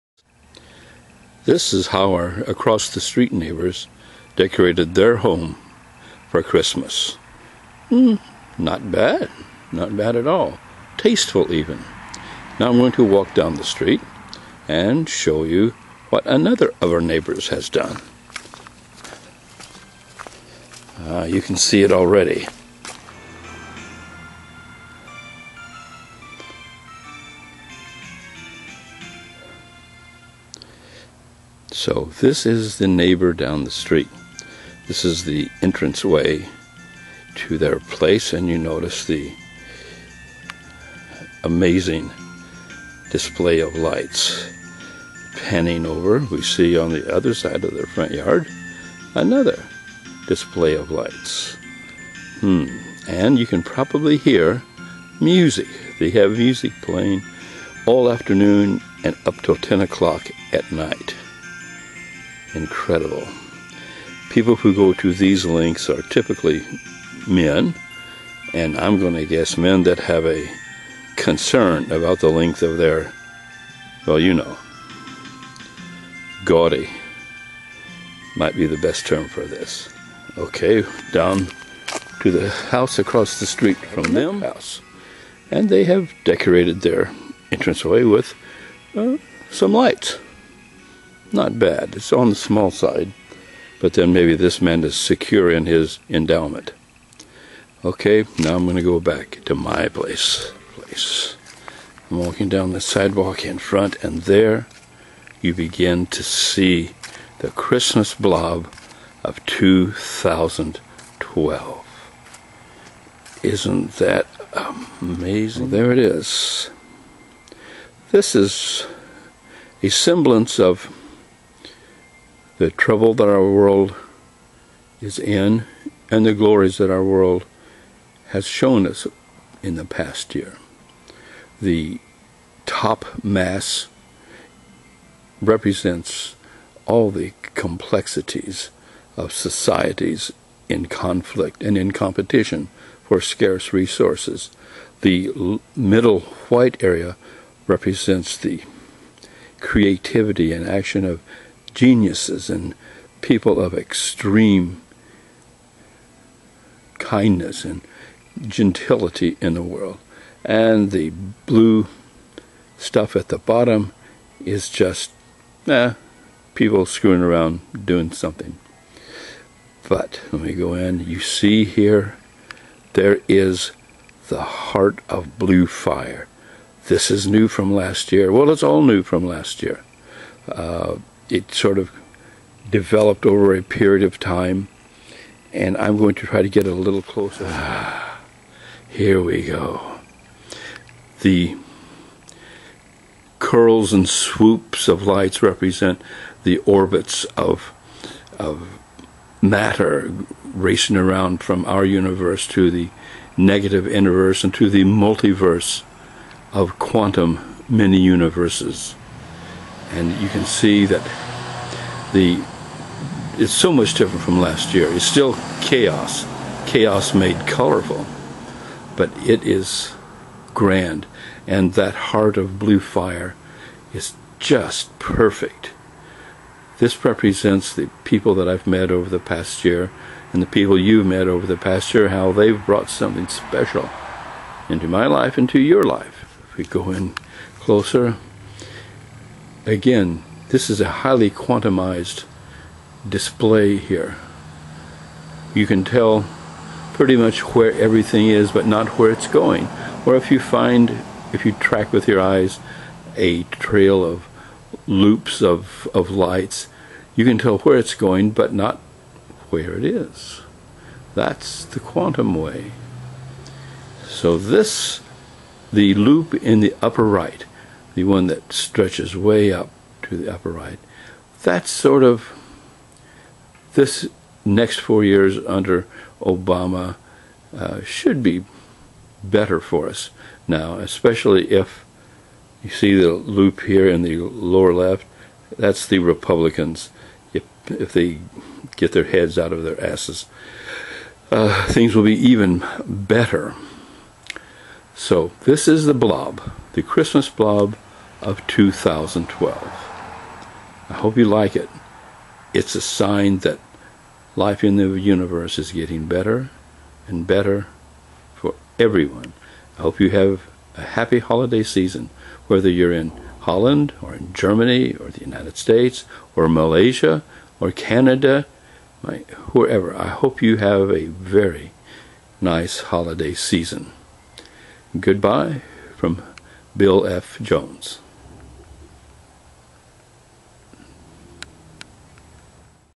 Xmas Blob of 2012
Free-form light sculpture and Quantum Theory model of the state of our world at Xmas.